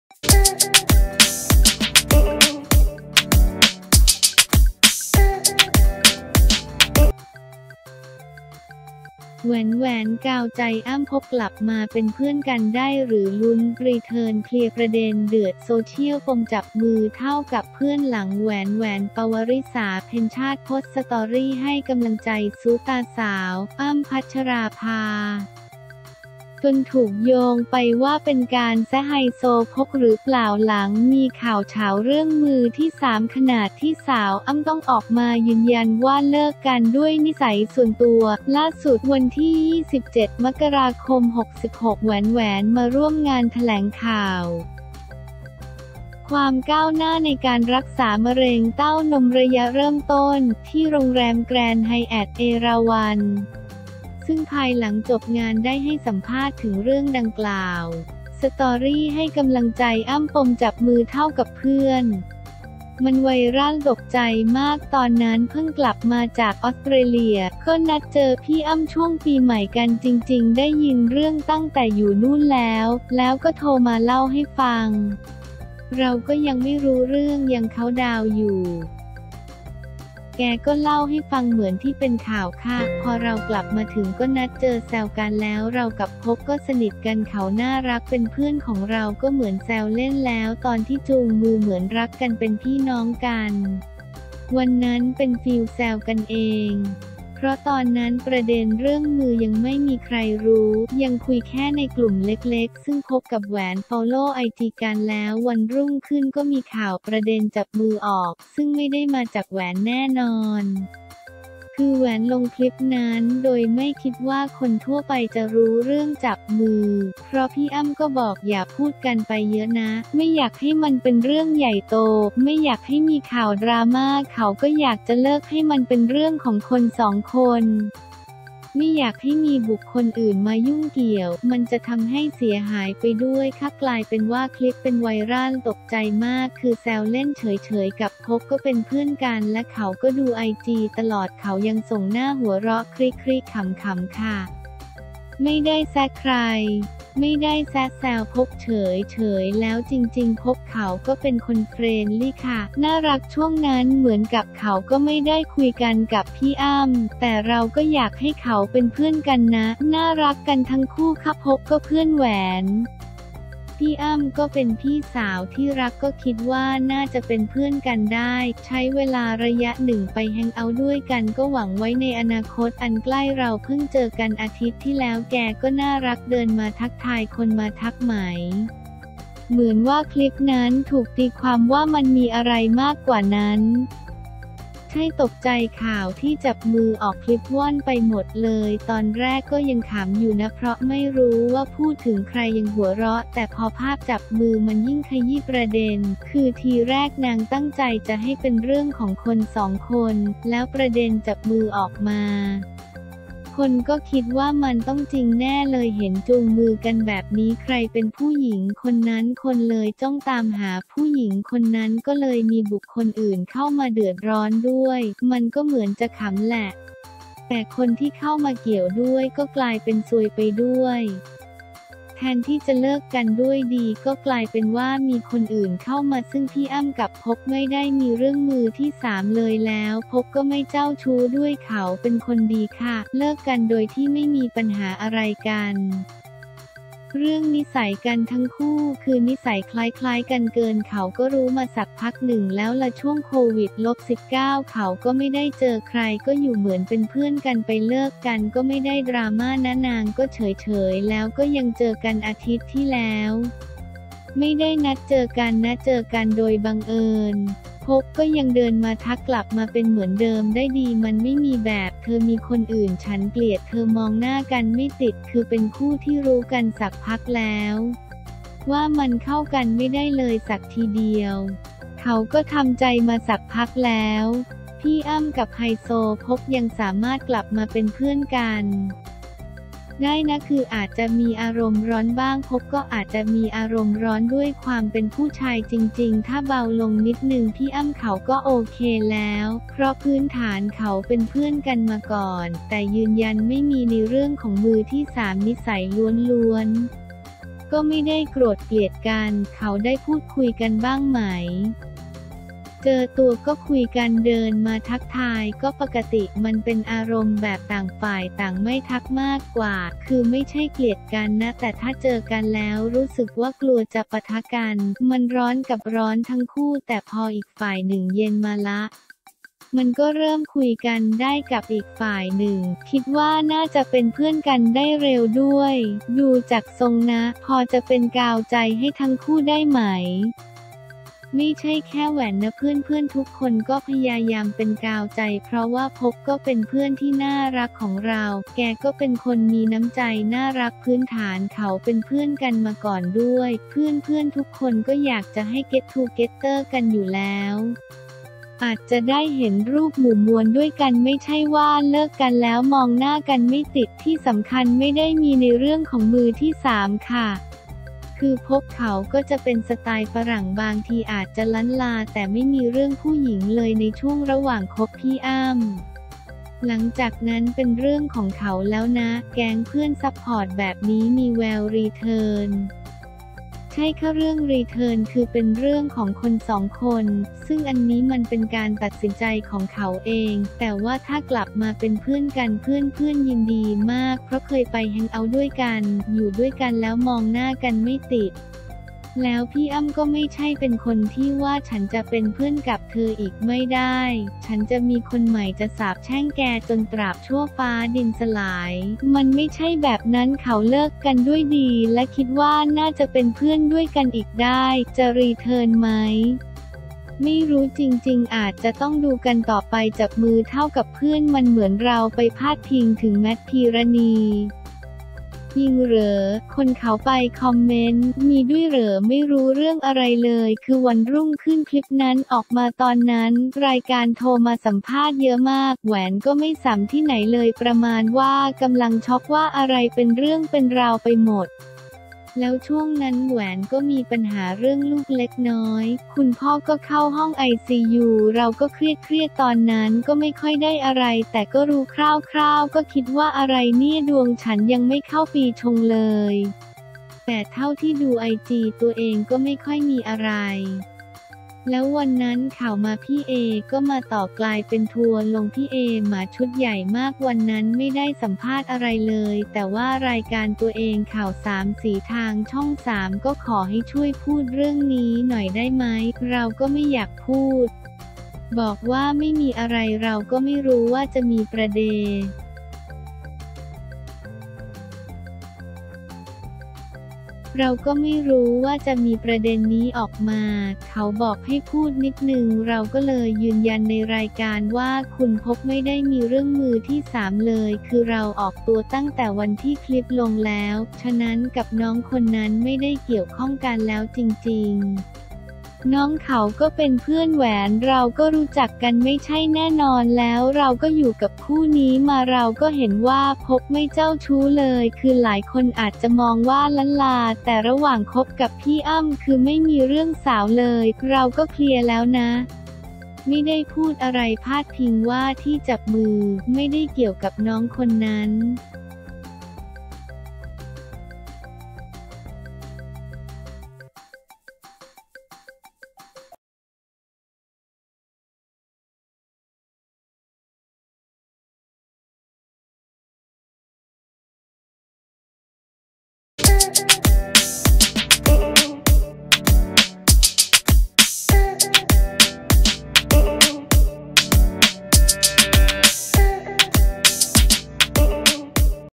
แหวนแหวน กาวใจ อั้ม พก กลับมาเป็นเพื่อนกันได้ หรือ ลุ้นรีเทิร์น
เคลียร์ประเด็นเดือดโซเชียล ปมจับมือ=เพื่อน หลัง แหวนแหวน ปวริศา เพ็ญชาติ โพสต์สตอรี่ให้กำลังใจซุป’ตาร์สาว อั้ม พัชราภา จนถูกโยงไปว่าเป็นการแซะ ไฮโซพก หรือเปล่า หลังมีข่าวฉาวเรื่องมือที่ 3 ขนาดที่สาวอั้ม ต้องออกมายืนยันว่าเลิกกันด้วยนิสัยส่วนตัว
ล่าสุด(27ม.ค.66) แหวนแหวน มาร่วมงานแถลงข่าว ความก้าวหน้าในการรักษามะเร็งเต้านมระยะเริ่มต้นฯ ที่ โรงแรมแกรนด์ไฮแอท เอราวัณ ซึ่งภายหลังจบงานได้ให้สัมภาษณ์ถึงเรื่องดังกล่าว
สตอรี่ให้กำลังใจอั้ม ปมจับมือ=เพื่อน? “มันไวรัล ตกใจมาก ตอนนั้นเพิ่งกลับมาจากออสเตรเลีย ก็นัดเจอพี่อั้มช่วงปีใหม่กัน จริงๆ ได้ยินเรื่องตั้งแต่อยู่นู่นแล้ว แล้วก็โทรมาเล่าให้ฟัง เราก็ยังไม่รู้เรื่อง ยังเคานต์ดาวน์อยู่ แกก็เล่าให้ฟังเหมือนที่เป็นข่าวค่ะ พอเรากลับมาถึงก็นัดเจอ แซวกัน

แล้วเรากับพกก็สนิทกัน เขาน่ารัก เป็นเพื่อนของเรา ก็เหมือนแซวเล่น แล้วตอนที่จูงมือ เหมือนรักกัน เป็นพี่น้องกัน วันนั้นเป็นฟีลแซวกันเอง เพราะตอนนั้นประเด็นเรื่องมือยังไม่มีใครรู้ ยังคุยแค่ในกลุ่มเล็กๆ ซึ่งพกกับแหวนฟอลโลว์ไอจีกัน แล้ววันรุ่งขึ้นก็มีข่าวประเด็นจับมือออก ซึ่งไม่ได้มาจากแหวนแน่นอน คือแหวนลงคลิปนั้นโดยไม่คิดว่าคนทั่วไปจะรู้เรื่องจับมือ
เพราะพี่อั้มก็บอกอย่าพูดกันไปเยอะนะ ไม่อยากให้มันเป็นเรื่องใหญ่โต ไม่อยากให้มีข่าวดราม่า เขาก็อยากจะเลิกให้มันเป็นเรื่องของคนสองคน ไม่อยากให้มีบุคคลอื่นมายุ่งเกี่ยว มันจะทำให้เสียหายไปด้วยค่ะ กลายเป็นว่าคลิปเป็นไวรัล ตกใจมาก คือแซวเล่นเฉยๆ กับพกก็เป็นเพื่อนกัน และเขาก็ดูไอจีตลอด เขายังส่งหน้าหัวเราะคริ คริ ขำๆ ค่ะ”

ไม่ได้แซะใคร? “ไม่ได้แซะ แซวพกเฉยๆ แล้วจริงๆพกเขาก็เป็นคนเฟรนด์ลี่ค่ะ น่ารัก ช่วงนั้นเหมือนกับเขาก็ไม่ได้คุยกันกับพี่อั้ม แต่เราก็อยากให้เขาเป็นเพื่อนกันนะ น่ารักกันทั้งคู่ค่ะ พกก็เพื่อนแหวน พี่อั้มก็เป็นพี่สาวที่รัก ก็คิดว่าน่าจะเป็นเพื่อนกันได้ ใช้เวลาระยะหนึ่ง ไปแฮ้งก์เอ้าต์ด้วยกัน ก็หวังไว้ในอนาคตอันใกล้ เราเพิ่งเจอกันอาทิตย์ที่แล้ว แกก็น่ารักเดินมาทักทาย
คนมาทักไหม เหมือนว่าคลิปนั้นถูกตีความว่ามันมีอะไรมากกว่านั้น? “ใช่ ตกใจ ข่าวที่จับมือออก คลิปว่อนไปหมดเลย ตอนแรกก็ยังขำอยู่นะ เพราะไม่รู้ว่าพูดถึงใคร ยังหัวเราะ แต่พอภาพจับมือมันยิ่งขยี้ประเด็น คือทีแรกนางตั้งใจจะให้เป็นเรื่องของคนสองคน แล้วประเด็นจับมือออกมา คนก็คิดว่ามันต้องจริงแน่เลย

เห็นจูงมือกันแบบนี้ ใครเป็นผู้หญิงคนนั้น คนเลยจ้องตามหาผู้หญิงคนนั้น ก็เลยมีบุคคลอื่นเข้ามาเดือดร้อนด้วย มันก็เหมือนจะขำแหละ แต่คนที่เข้ามาเกี่ยวด้วย ก็กลายเป็นซวยไปด้วย แทนที่จะเลิกกันด้วยดี ก็กลายเป็นว่ามีคนอื่นเข้ามา ซึ่งพี่อั้มกับพกไม่ได้มีเรื่องมือที่ 3 เลย แล้วพกก็ไม่เจ้าชู้ด้วย เขาเป็นคนดีค่ะ”